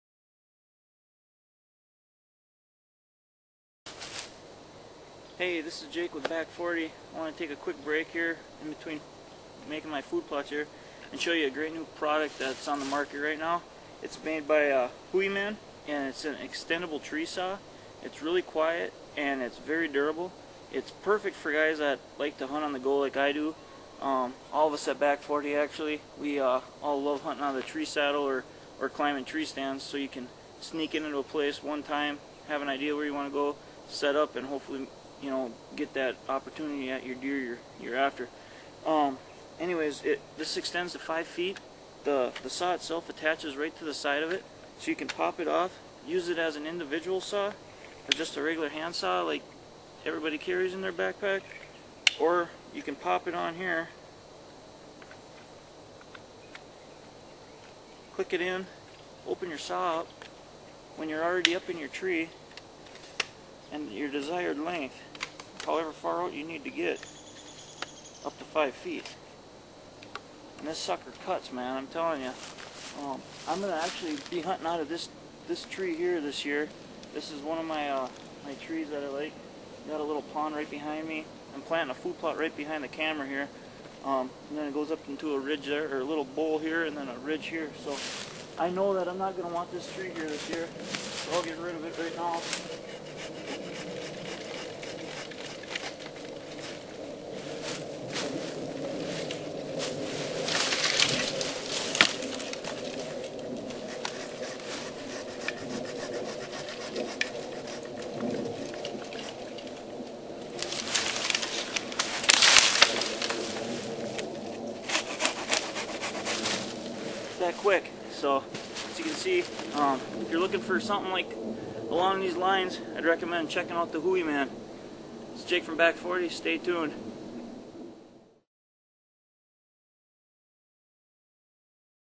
Back 40 outdoors product review
Back 40 Outdoors does a quick product review on the Hooyman saw